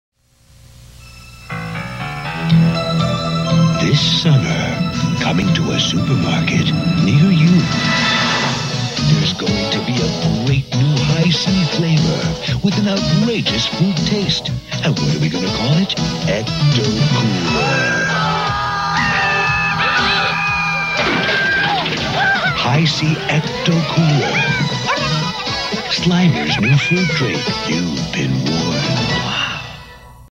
(1989) Hi-C Ecto Cooler Commercial
Ecto Cooler IN HD! I uploaded this while drinking a can of it, and it's just as delicious as I remember. I had it when I was little, then it was called Crazy Citrus Cooler and Shoutin' Orange Tangergreen. It's back in 2016, so you should probably go get some!